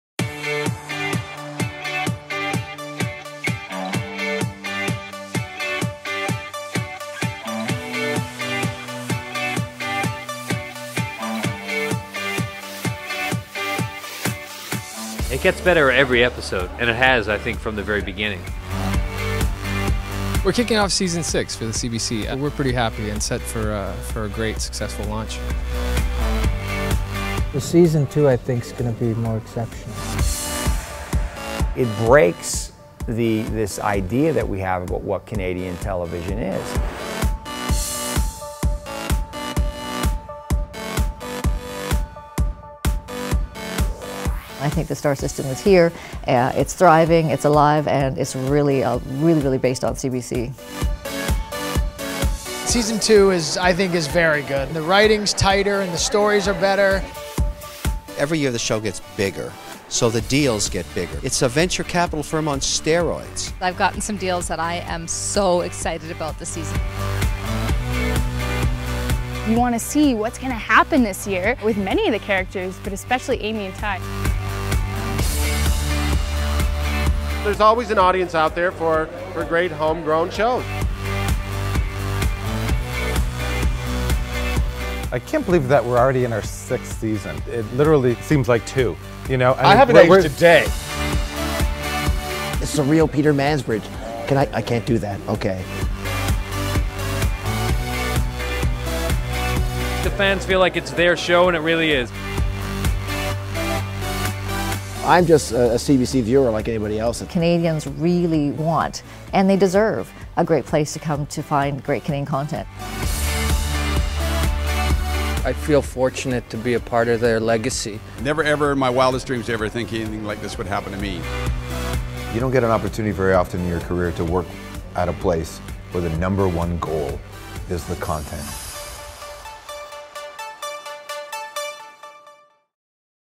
CBC Stars Sizzle at Winter Preview
Today (November 13), the stars of CBC's Winter season descended on the Toronto Broadcast Centre to preview their upcoming shows and we looked back at some of the best bits from the past year. Here are some of CBC Live's highlights.